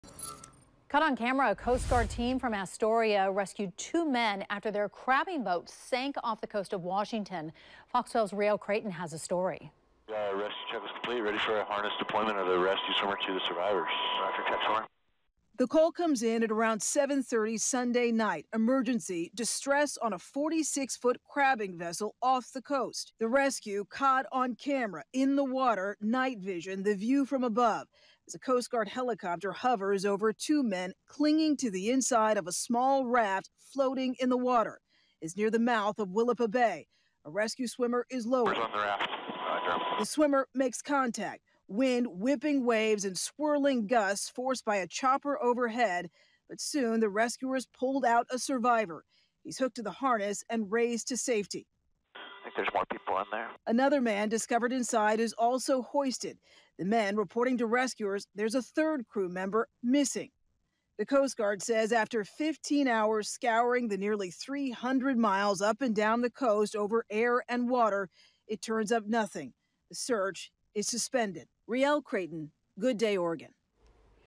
1 missing, 2 rescued from crab boat off Washington coast
A crew member remains missing and two others were rescued from crab boat that sank near Willapa Bay in southwest Washington on Sunday evening.